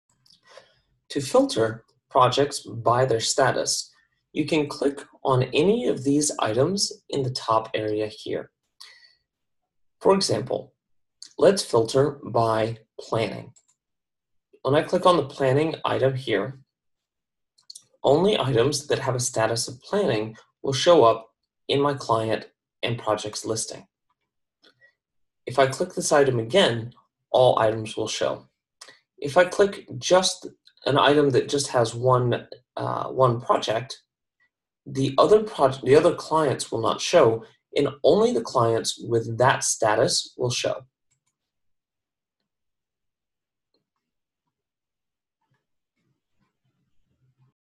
Filtering projects by status on the BrainLeaf dashboard
How to filter projects by their statuses on the BrainLeaf dashboard.